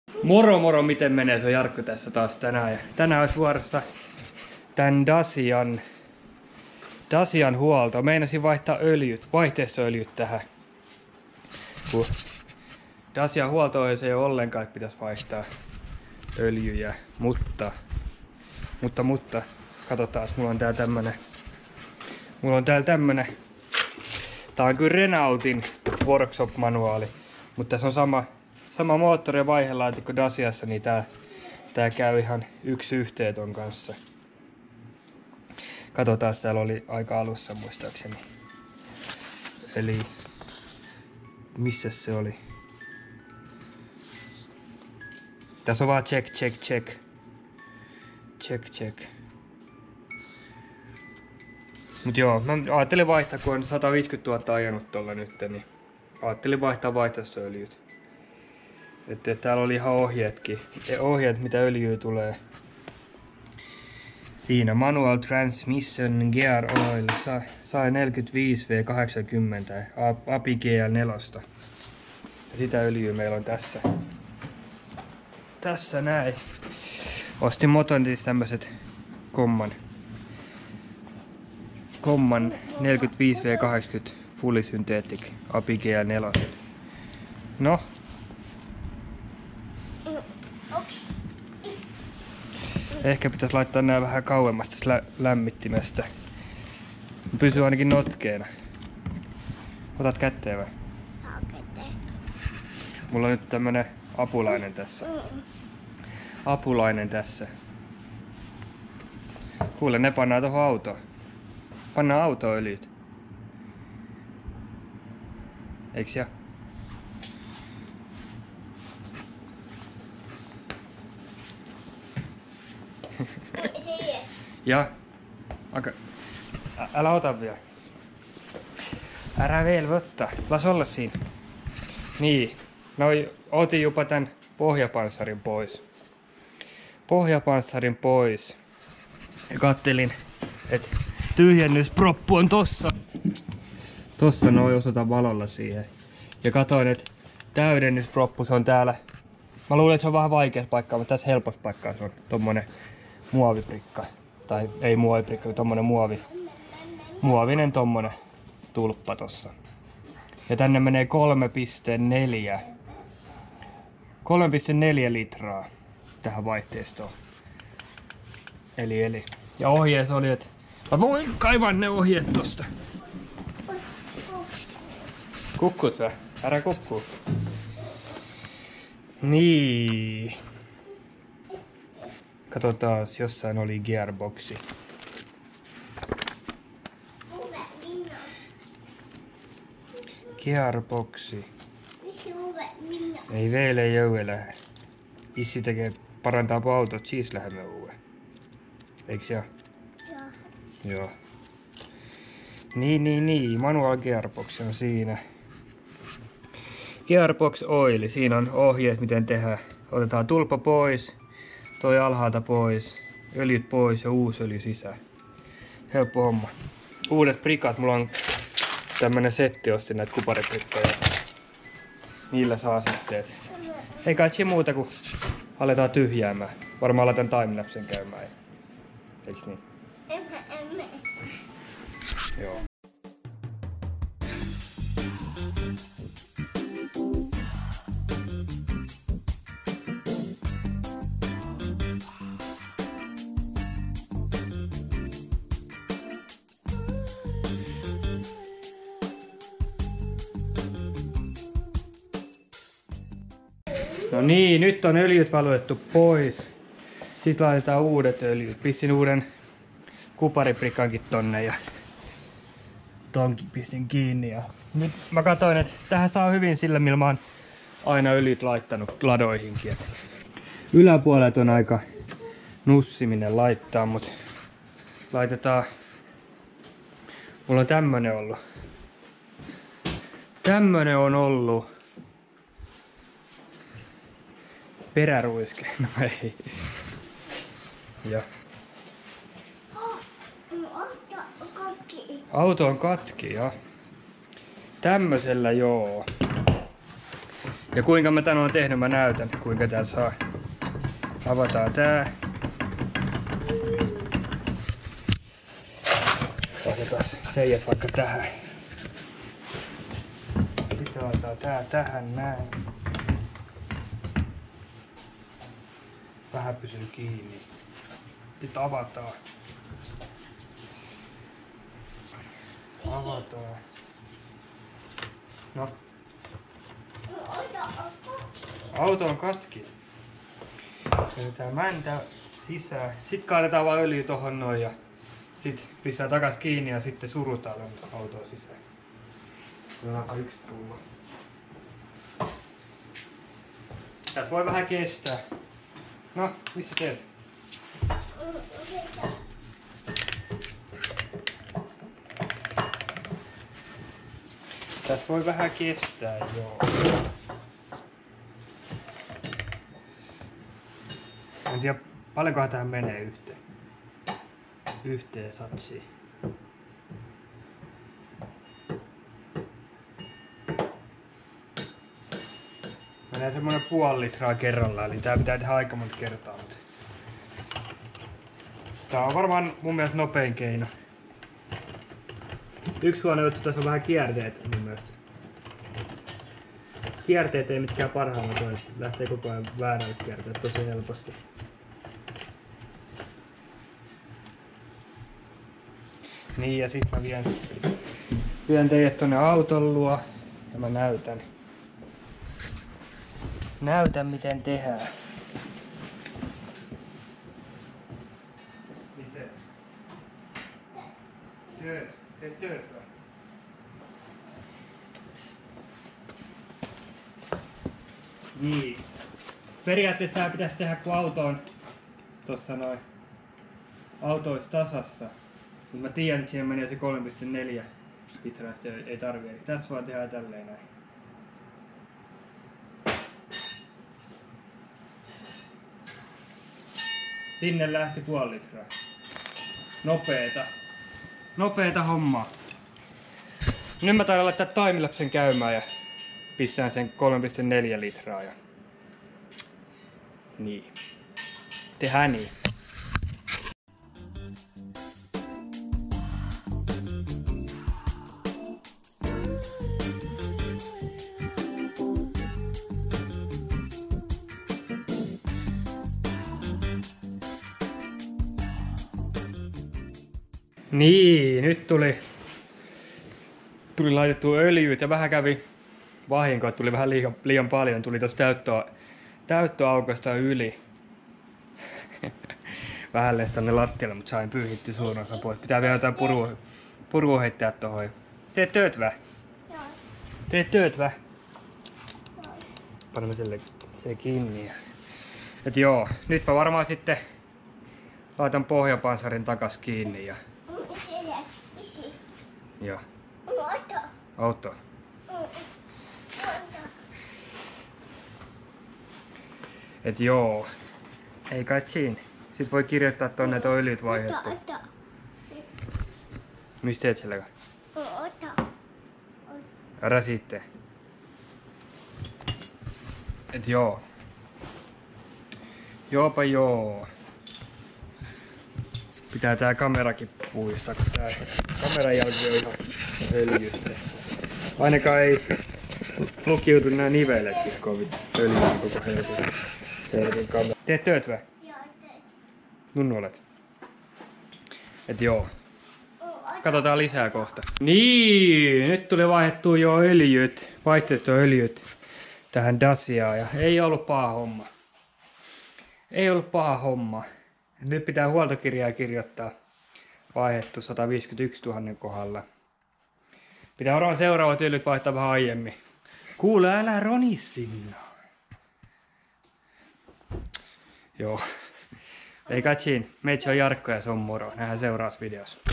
Vaihteistoöljyn vaihto Dacia Logan MCV -18
Vaihoin Daciaa vaihteistoöljyt